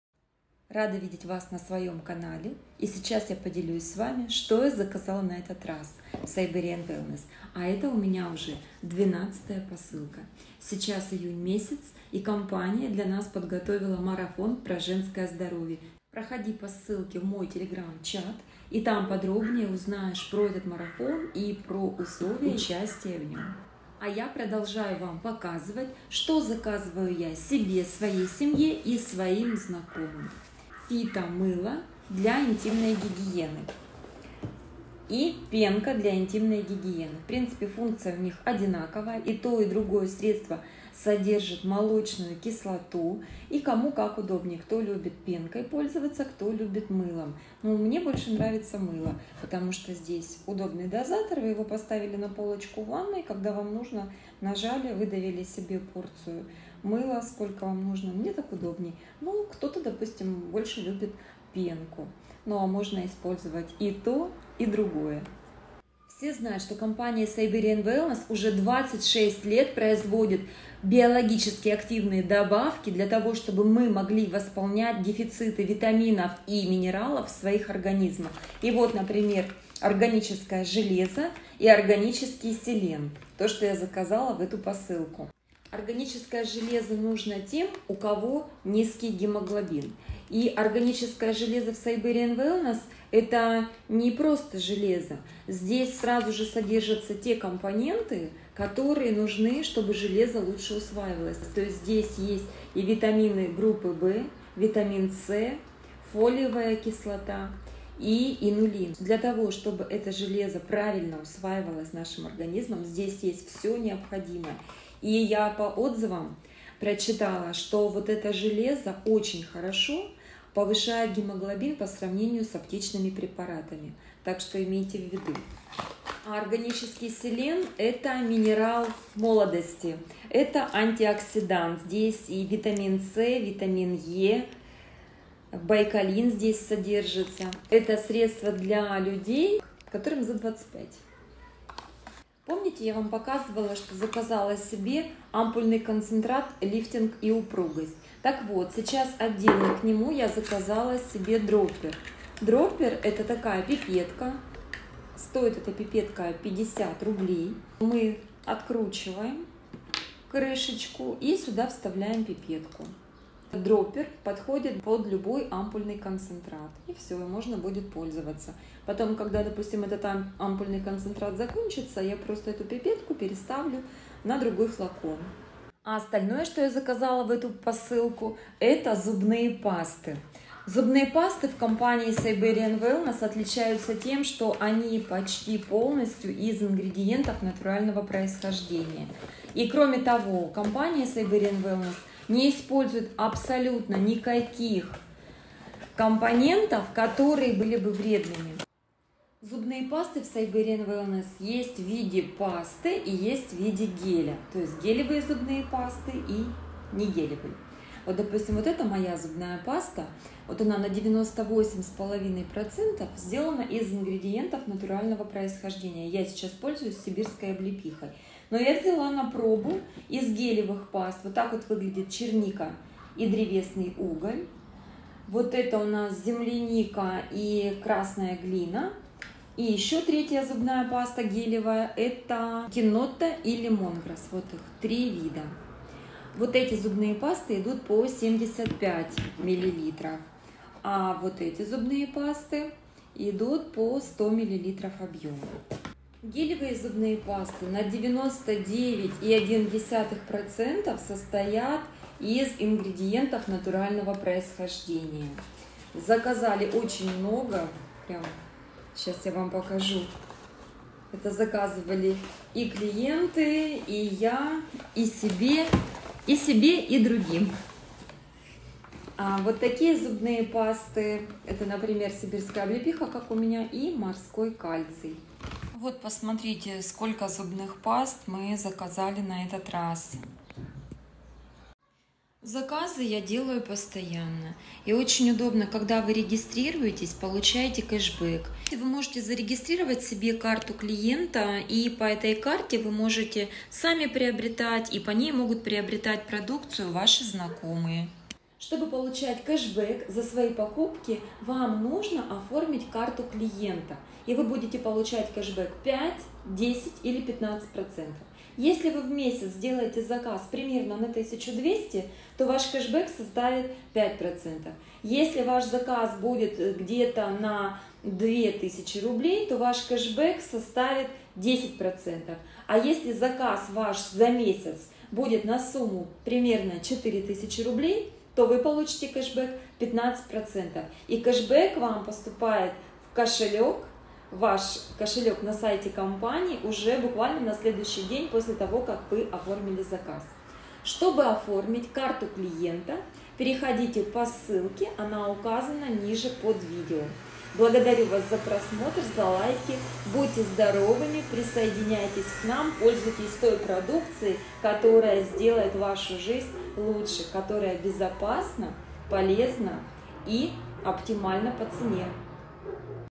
РАСПАКОВКА 12. SIBERIAN WELLNESS, Сибирское Здоровье. Маргарита Воронина. Славянск-на-Кубани
Мой 12 заказ, получила 9 июня 2023

Оформить КАРТУ КЛИЕНТА и получать кешбек 5-10-15% по ссылке

Вступай!

❤️ЗАХОДИ НА КАНАЛ В ТЕЛЕГРАММЕ О КРАСОТЕ, ЗДОРОВЬЕ И ДОХОДЕ